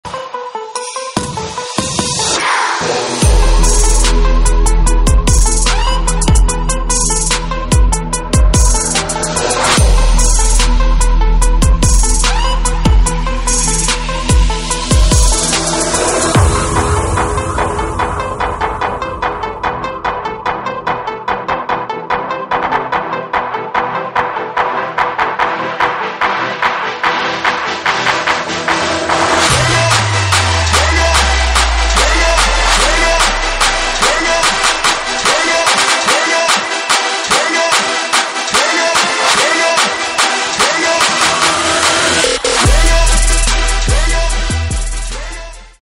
Funny Lifted Honda Hatchback
Funny lifted Honda that went mudding before the show. I will upload a video atleast every month.